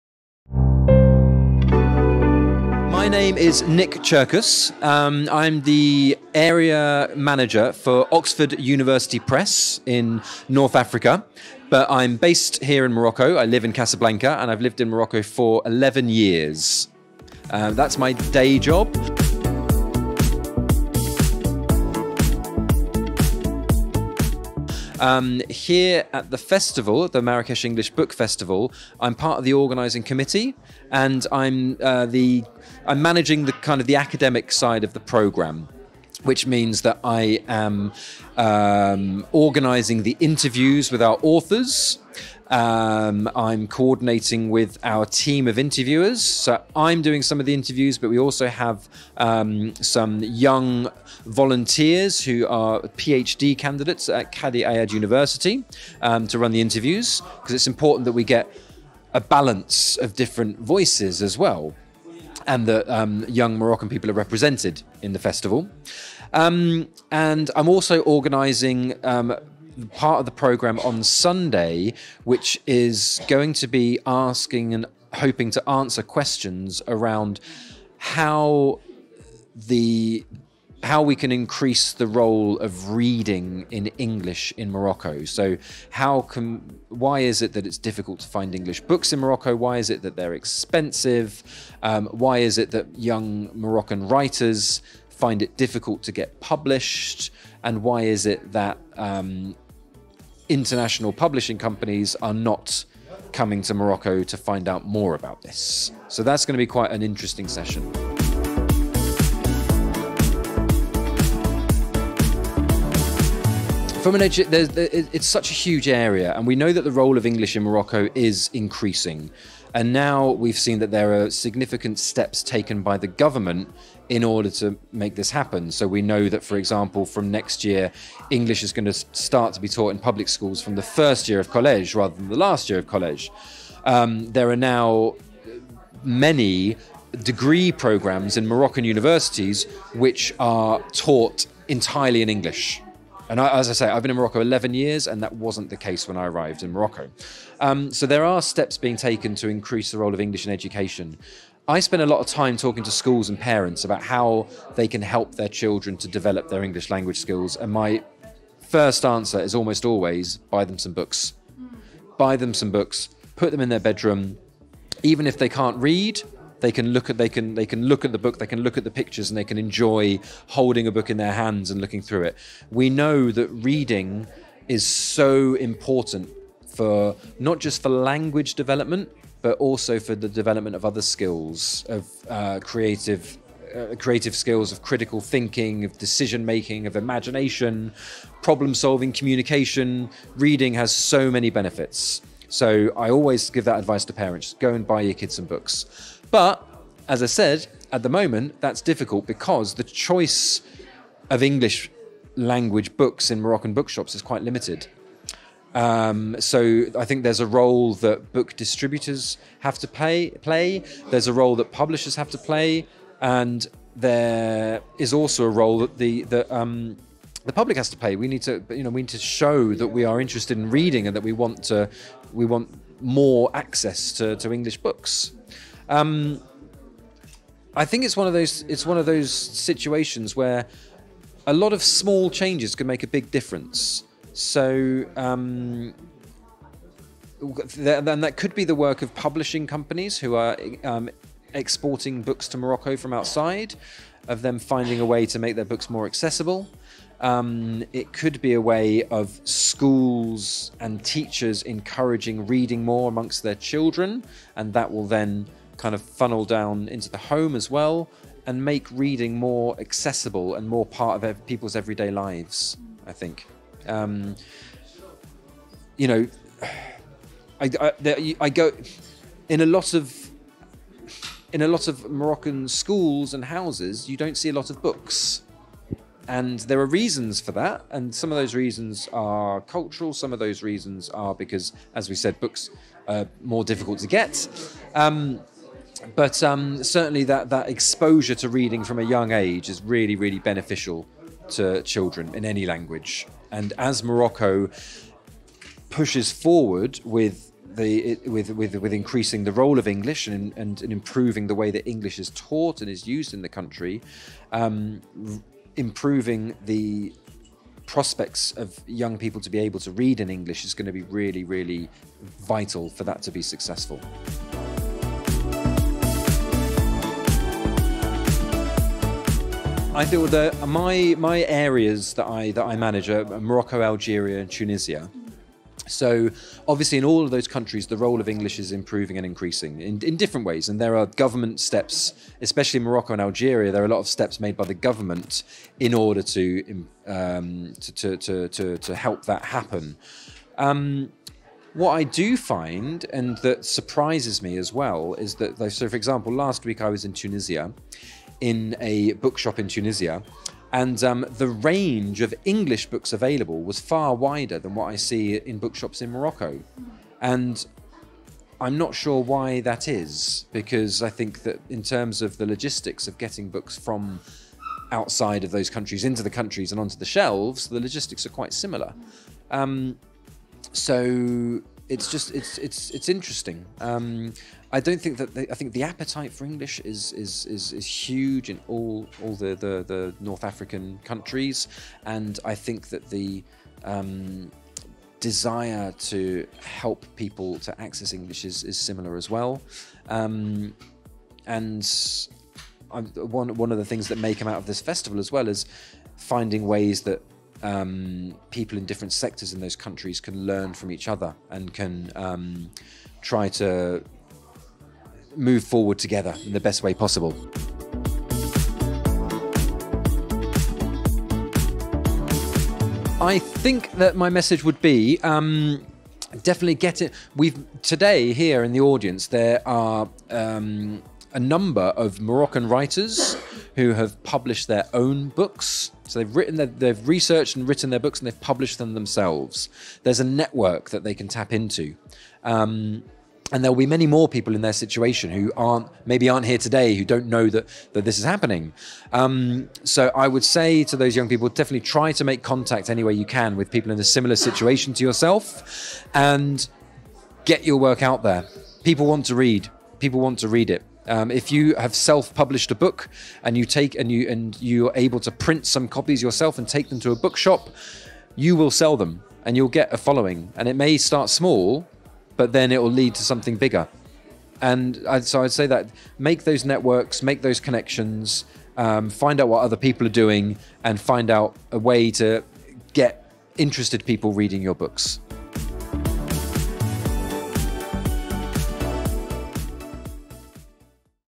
Morocco’s first English book festival reflects social, governmental will
Moroccan English readers, writers, and publishing professionals gathered at the Marrakech English Book Festival (MEBF) this weekend to discuss the future of the language in the Kingdom, in light of domestic language trends.

In recent years, there has been a major shift in many young Moroccans’ foreign language preferences, which has slowly moved throughout the years from French to English.

Although the former still reigns supreme, the Moroccan people and their government are indeed attempting a slow transition towards a more anglophone country and population.

Hespress English.. Reporting on what matters.